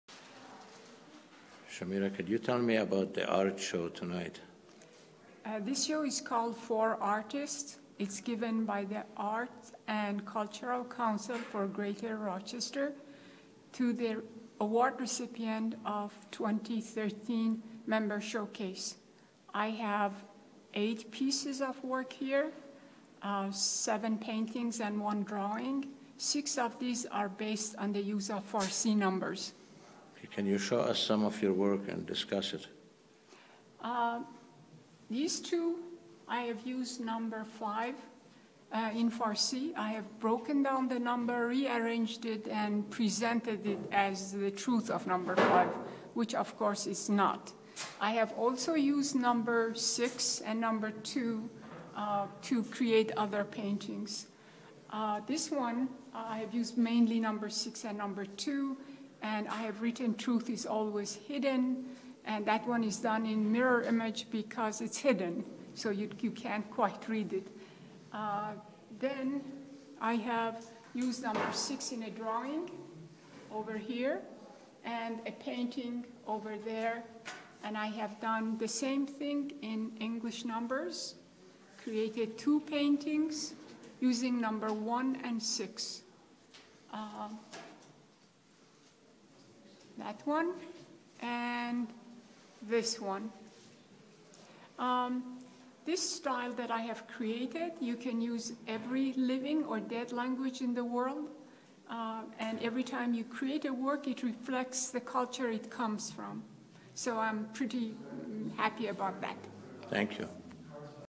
Iranian Contemporary Painting
Contemporary Iranian artist Shamira Derakhshani Nicolas describes her artwork, which involves the use of numbers in Farsi and other languages.